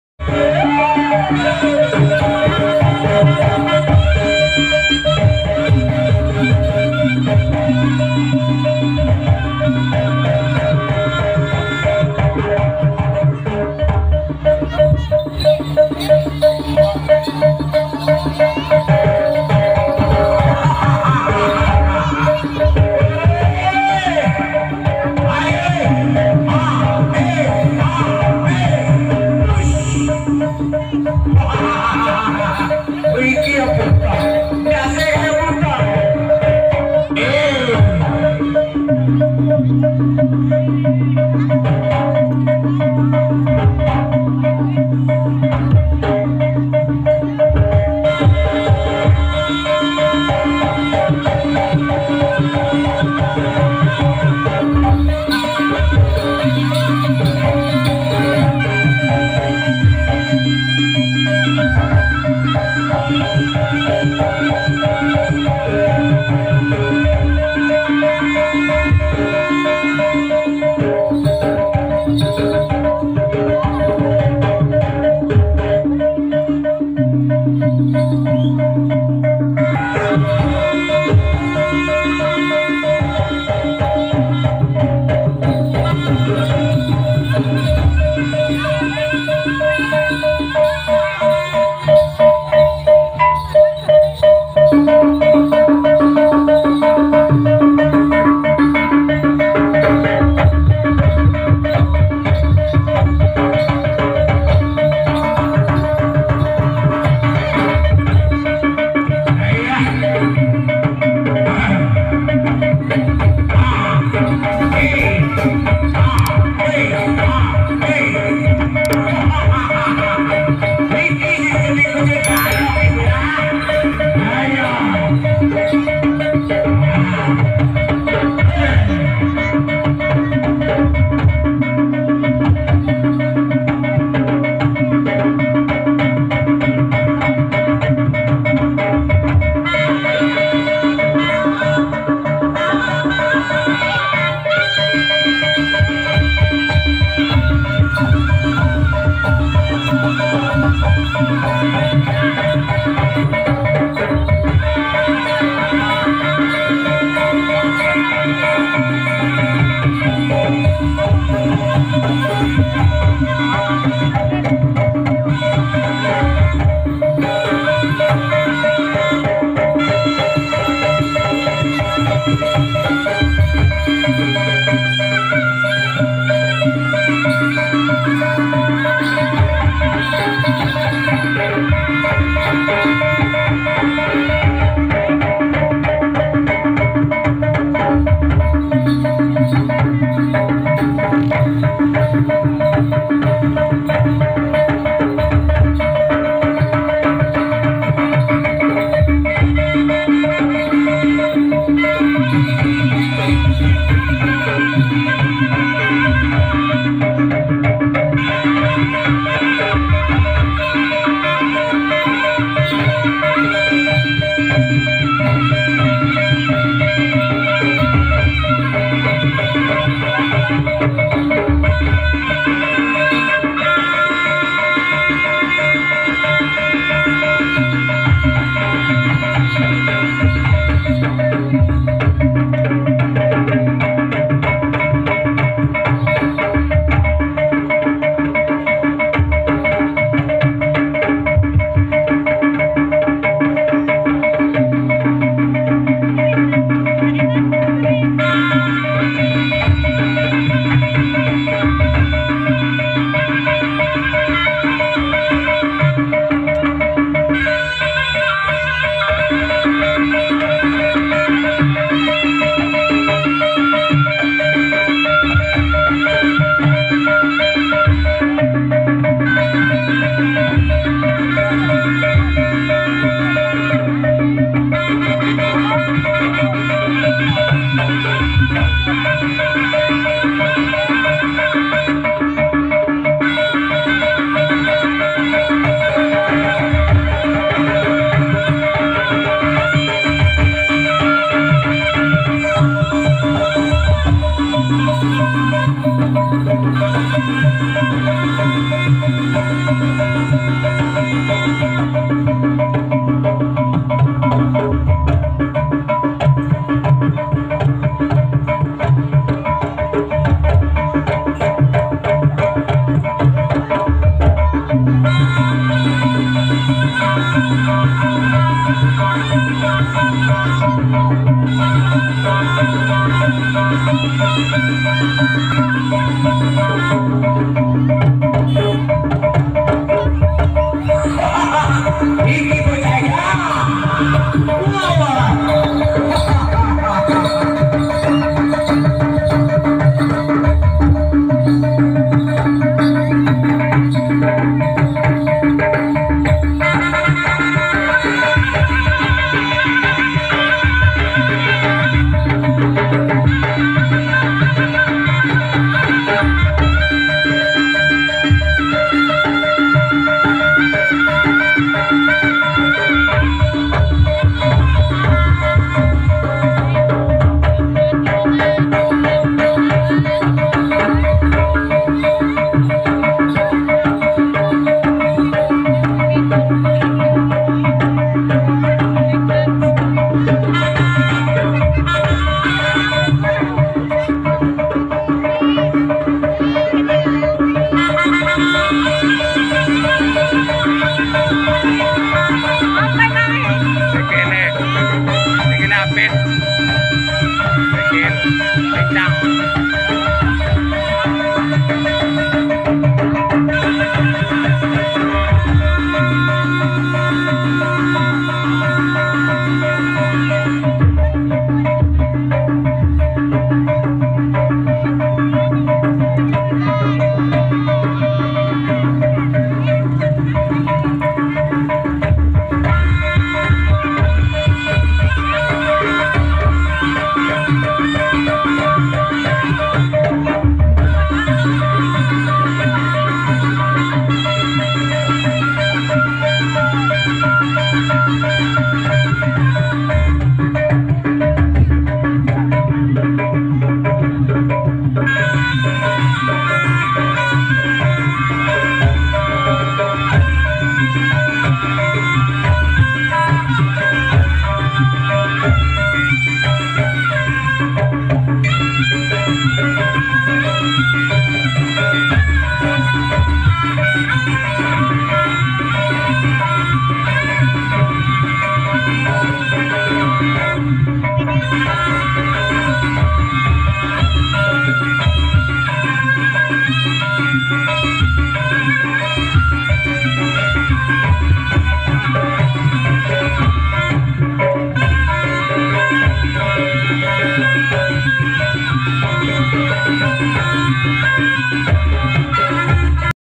KUCINGAN LANANG CARI BETINA ||krido iromo di mujirahayu
JARANAN KUCINGAN LANANG CARI BETINA ||krido iromo di mujirahayu#jarananlampung

JARANAN KRIDO IROMO BERASAL PIMPINAN BPK KADRI BERALAMTKAN DI SIMPANG AGUNG KECAMATAN SEPUTIH AGUNG LAMPUNG TENGAH